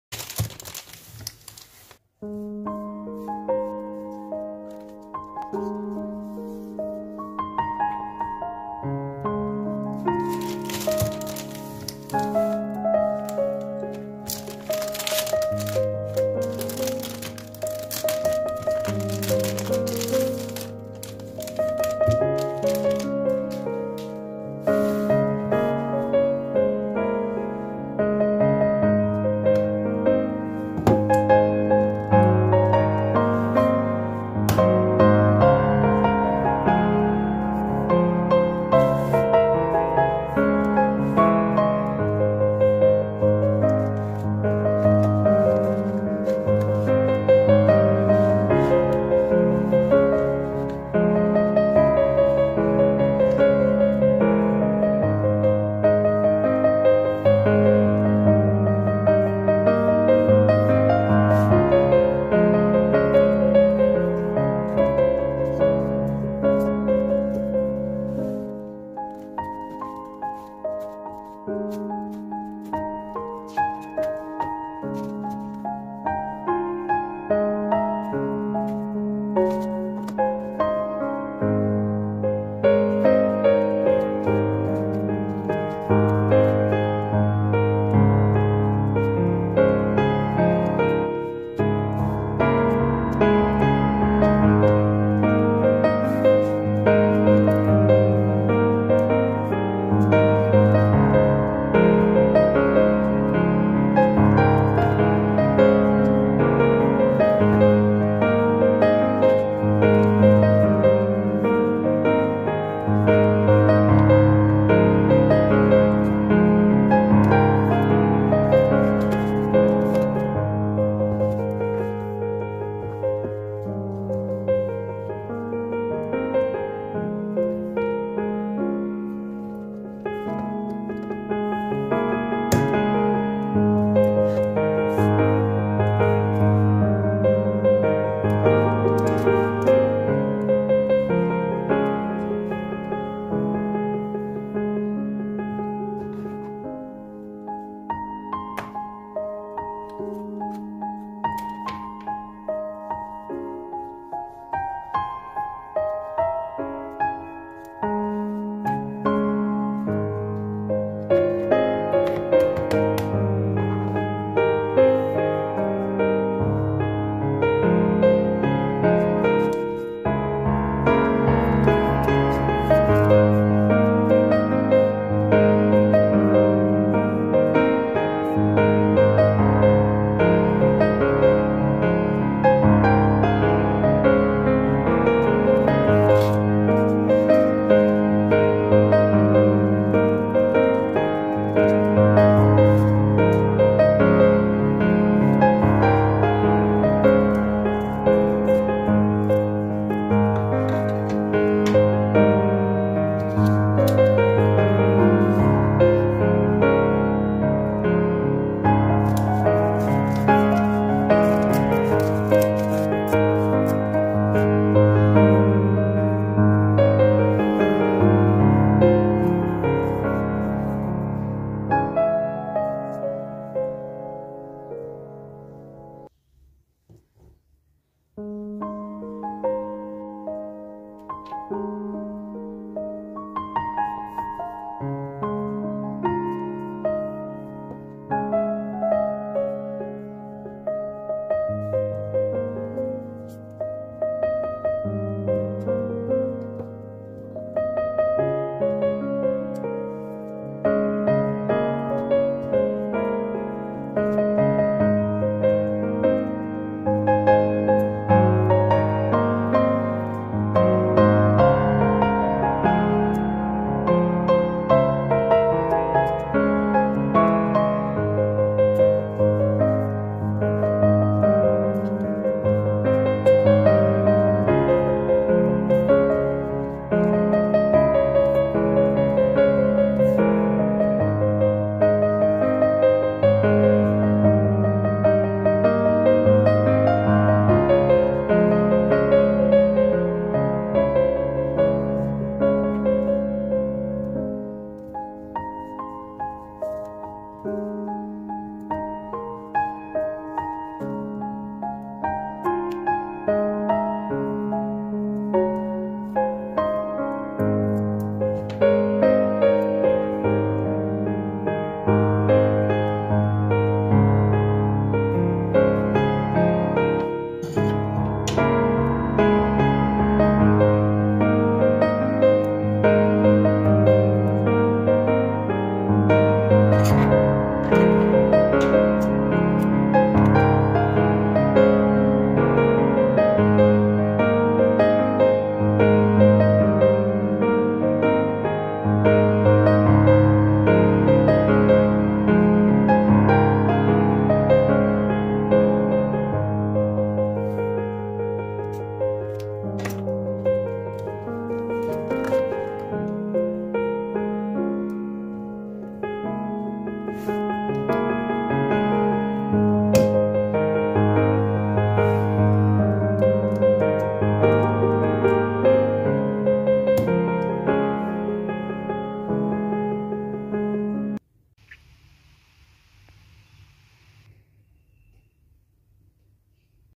DECORATING MY A6 RING BINDER | JOURNAL WITH ME
☁️Hi friends ^⌒★

☁️How is everyone doing? I got an A6 ring binder so I wanted to make a video about setting it up. I may do a journal with me video later in this journal. I hope everyone is safe and healthy:) 🤍